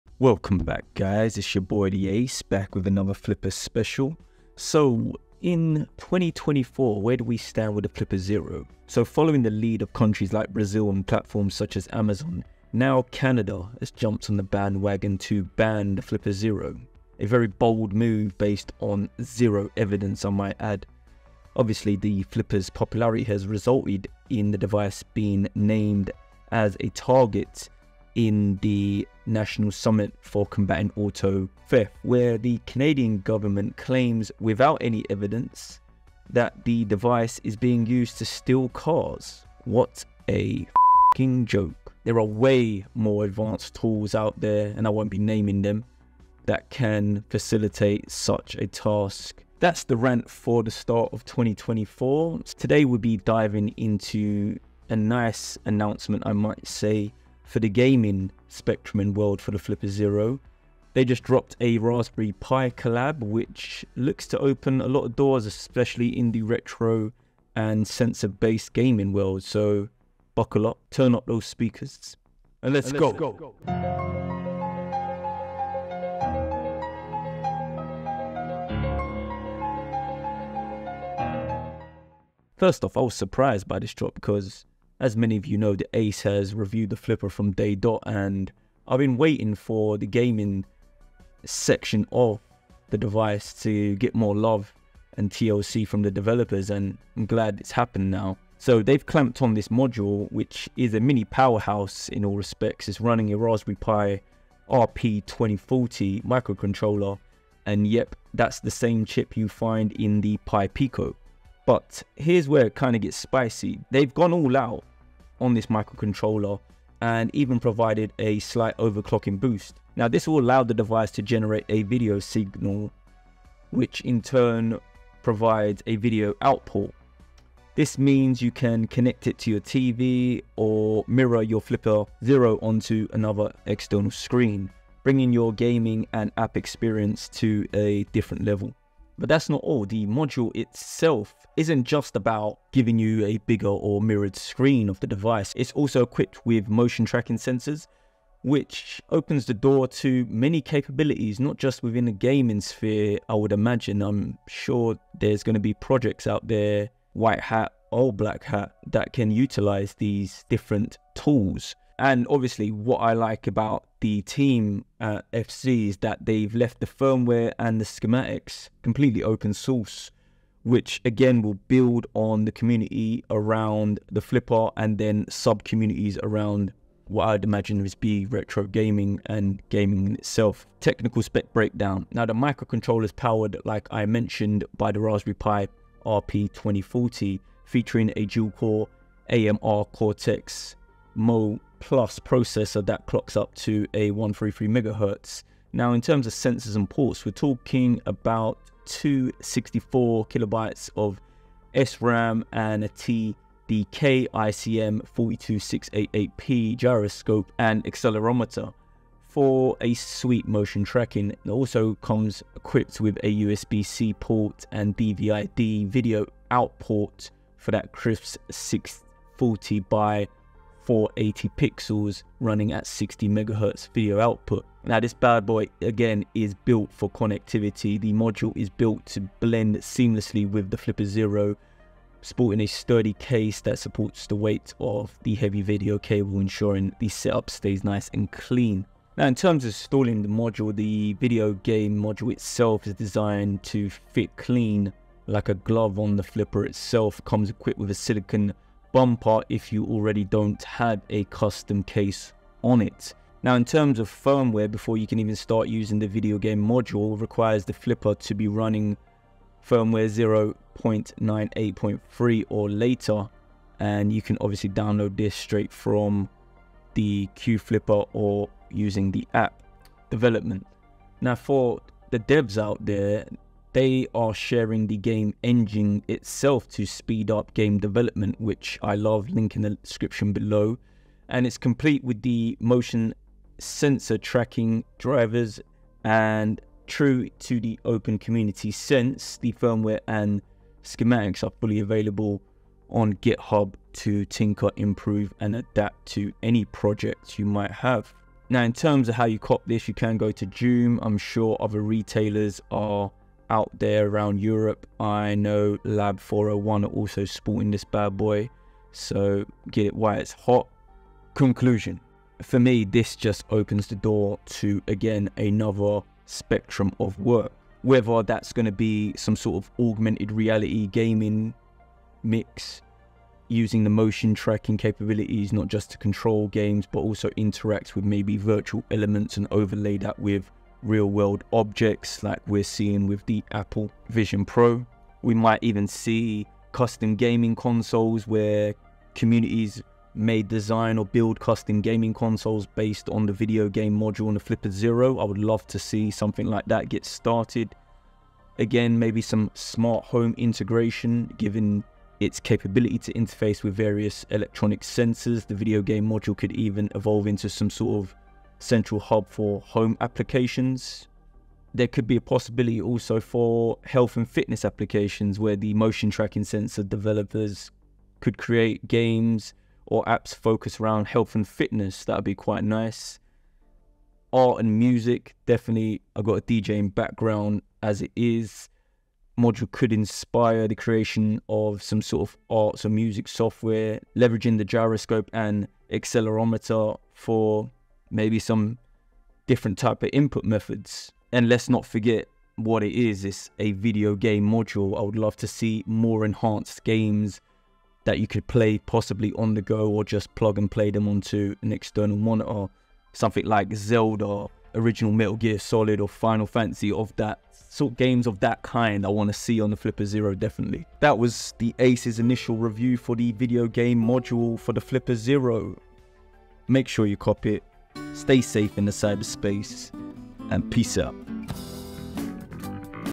Flipper Zero - Video Game Module
Today, we're taking a special look at the newest addition to the Flipper Zero ecosystem - The Video Game Module, released as only the second official module after the WiFi dev board. In this video, I unravel every aspect of this intriguing mod. Is it the next must-have for tech enthusiasts and gamers alike? Let's find out! 🎮🔍
✅ Invest in skills!